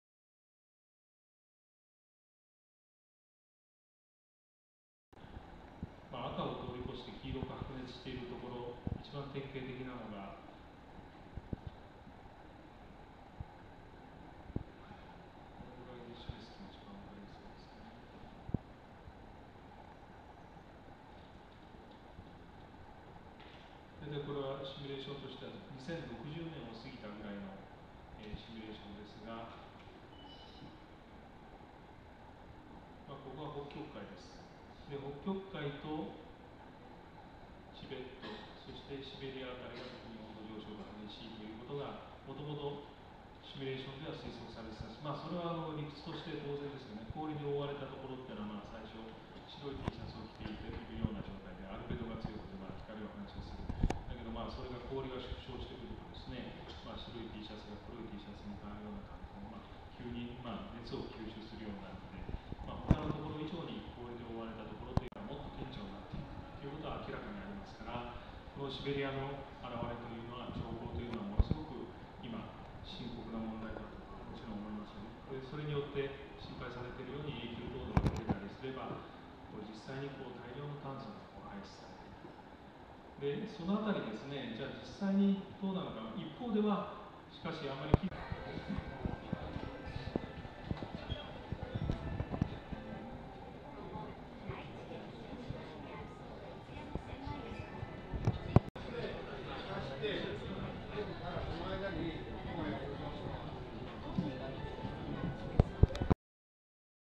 ＣＯＰ１０_2010NAGOYA
今日（10月18日）から開催されたCOP10へ行ってきました。
たくさんの国の方たちはいましたが、会場内は人がまばら。。。
全然盛り上がっていない。